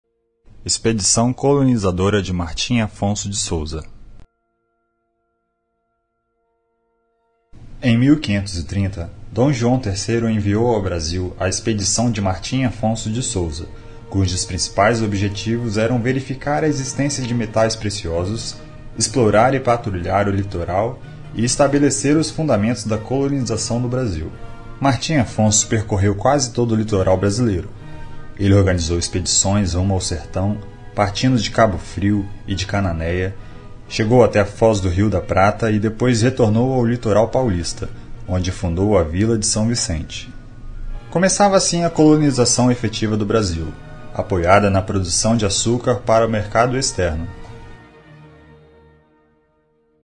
Expedição Colonizadora de Martin Afonso de Souza (animação 03)
Animação realizada para o curso de Pós-graduação de Gestão Pública Municipal do Cead - Ifes. A equipe de produção contou com Juliana Cristina e Aline Freitas (direção de produção), Elton Vinícius Silva (direção/montagem), Andreia Carvalho (animação/direção de fotografia), Ana Elisa Poubel (animação), Carlos Henrique Morelato (locução), Tiago Corrente (áudio).